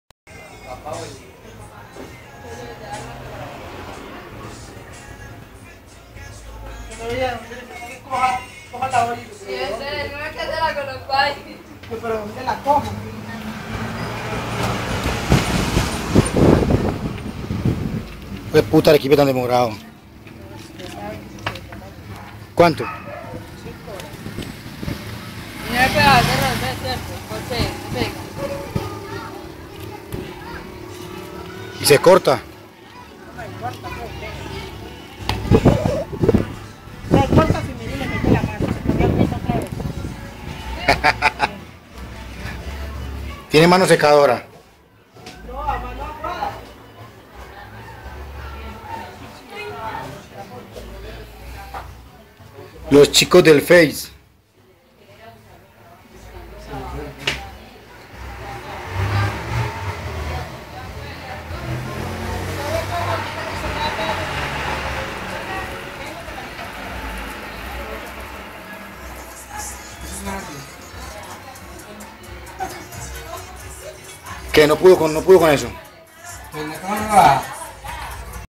AREQUIPE 2011 CARTAGO VALLE
UN BOLEO EL HPTA 6 HORAS DE REVUELTA HUFFFFFF QUE CALOR